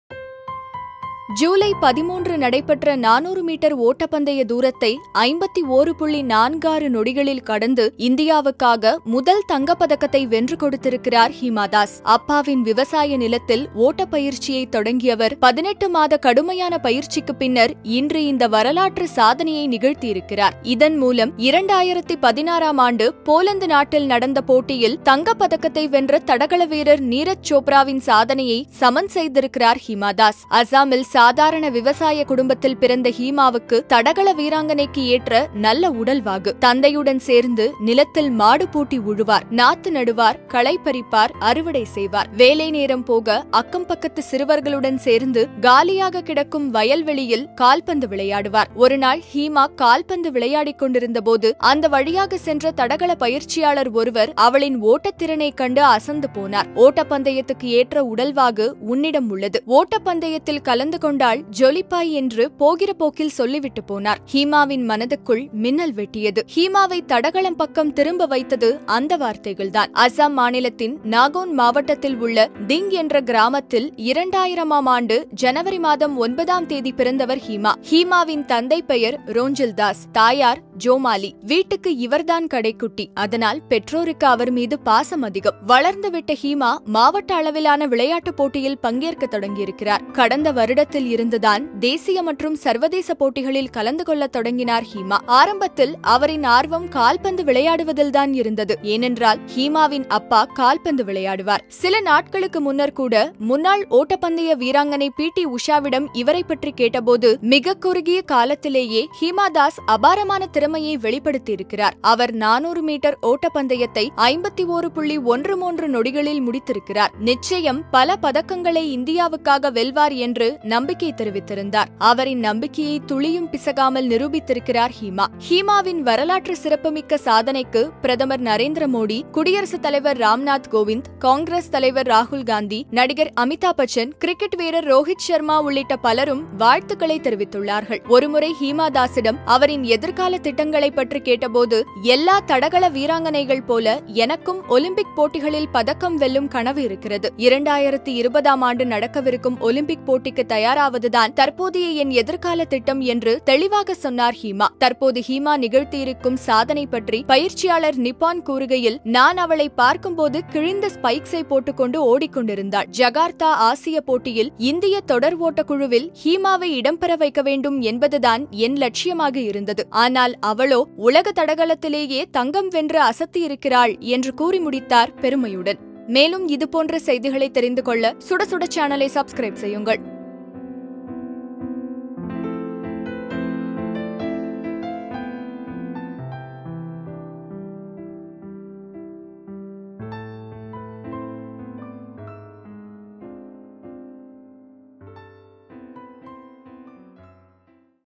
யார் இந்த ஹீமா தாஸ்? | HimaDas
சத்தமே இல்லாமல் பின்லாந்தில் நடந்துகொண்டிருக்கும் உலக U-20 தடகள சாம்பியன்ஷிப்பில் இந்தியாவுக்குப் பெருமை சேர்ந்திருக்கிறார், அஸ்ஸாம் மாநிலத்தைச் சேர்ந்த 18 வயது தடகள வீராங்கனை , ஹீமா தாஸ்.400 மீட்டர் ஒட்டப்பந்தய தூரத்தை, 51.46 நொடிகளில் கடந்து, இந்தியாவுக்காக முதல் தங்கப் பதக்கத்தை வென்றிருக்கிறார் ஹீமா தாஸ்.

who is  hima das ?